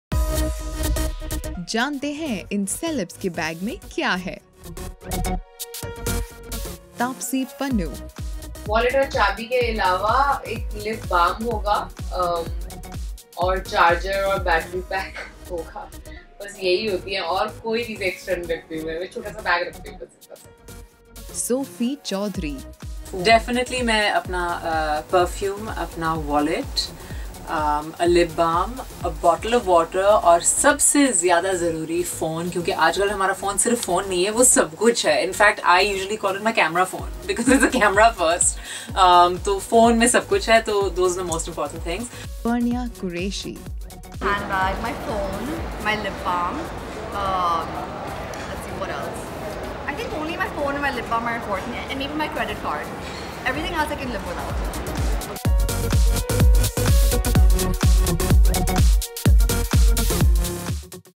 जानते है इन सेलेब्स के बैग में क्या क्या हैं - Onlymyhealth.com
इन एक्ट्रेस के हैंडबैग्स में क्या है क्या ये रखती है घर से बहार निकलने से पहले